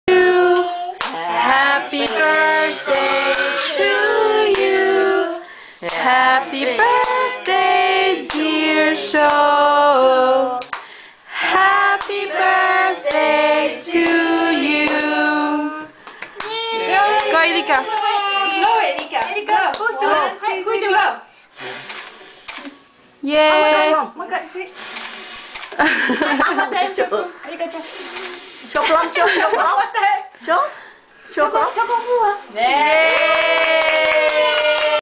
Shou's 1st Birthday
Blowing his first candle with Elika's assistance